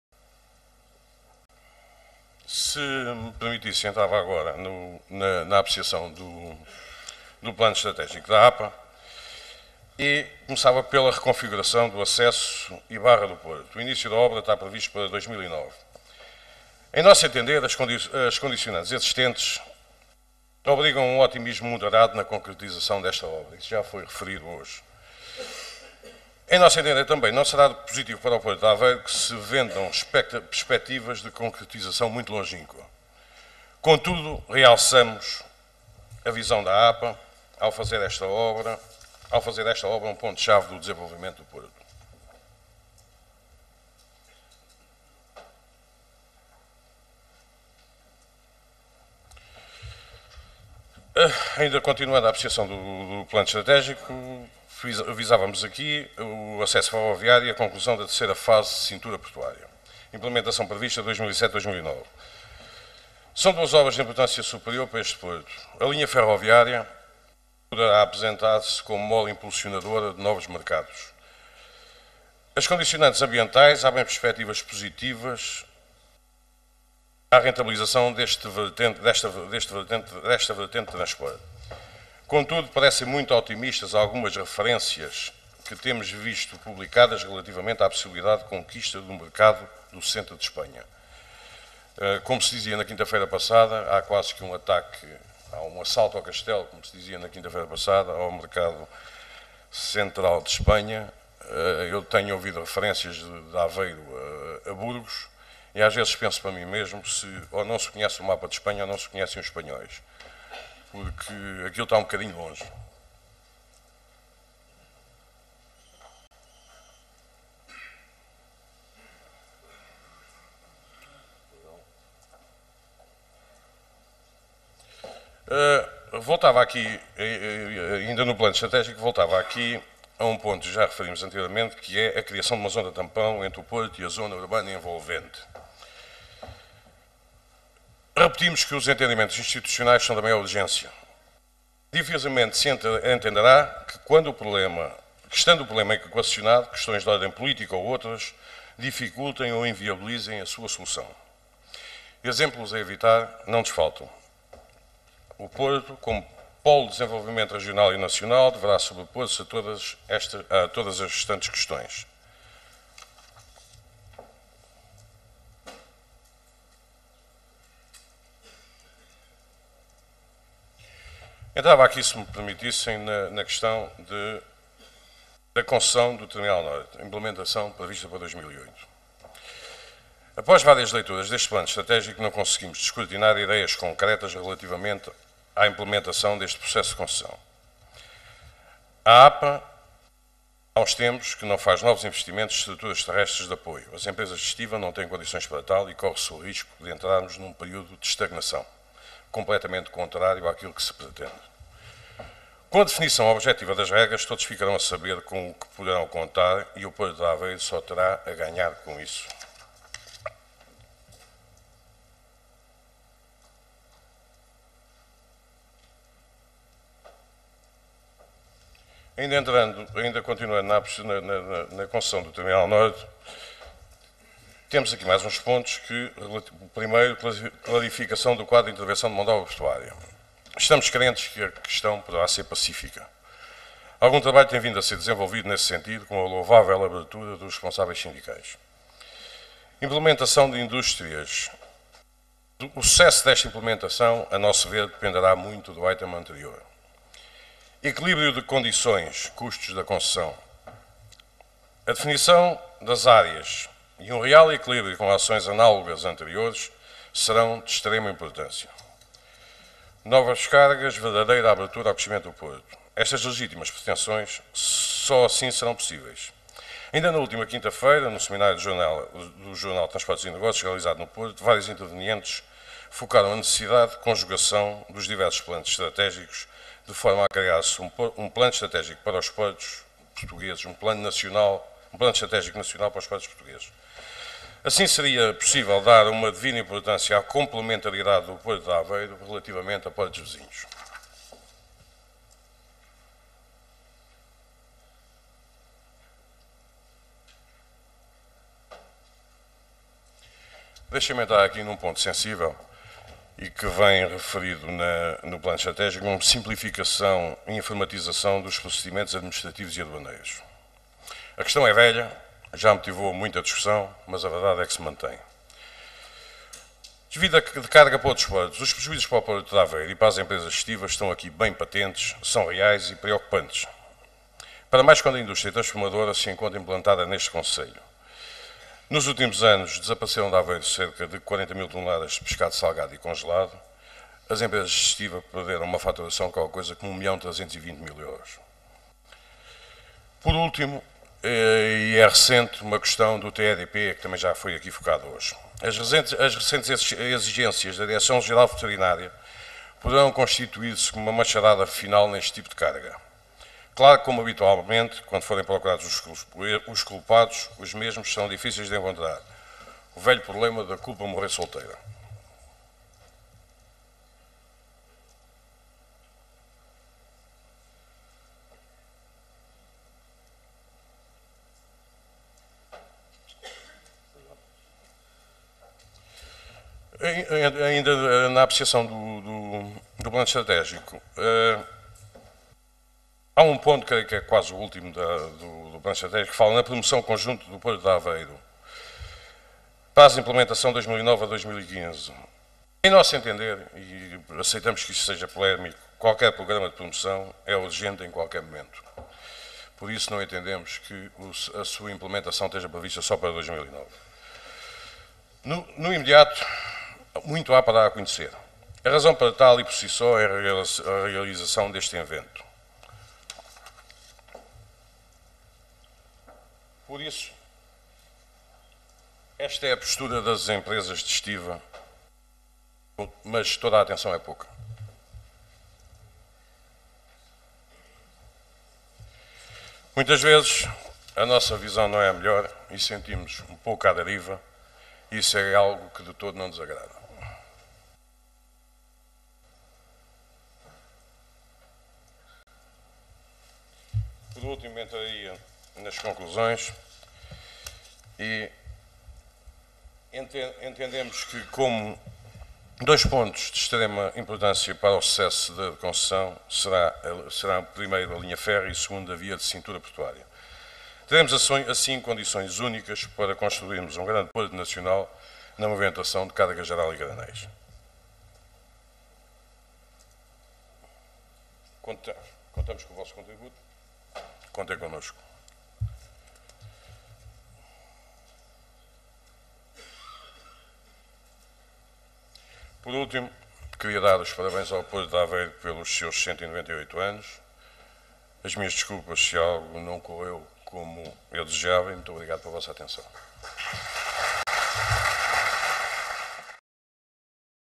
Porto de Aveiro: Estratégia e Futuro (XVI) -- Lourenço Lopes
Sessão de apresentação do Plano Estratégico do Porto de Aveiro, realizada a 3 de Abril de 2006 no Museu Marítimo de Ílhavo.

Segundo Painel com moderação da Dra. Elisabete Rita, Secretária-Geral da Associação Industrial do Distrito de Aveiro.

Segundo e último excerto da intervenção de José Manuel Lourenço Lopes, representante da AEEPA (Associação das Empresas de Estiva do Porto de Aveiro), subordinada ao tema "A Visão Estratégica das Empresas de Estiva".

Esta apresentação à Comunidade Portuária de Aveiro, efectuada no colóquio "Porto de Aveiro: Estratégia e Futuro", destinou-se, também, a assinalar a institucionalização do Dia do Porto de Aveiro, que passou, a partir de 2006, a celebrar-se a 3 de Abril de cada ano.

Museu Marítimo de Ílhavo, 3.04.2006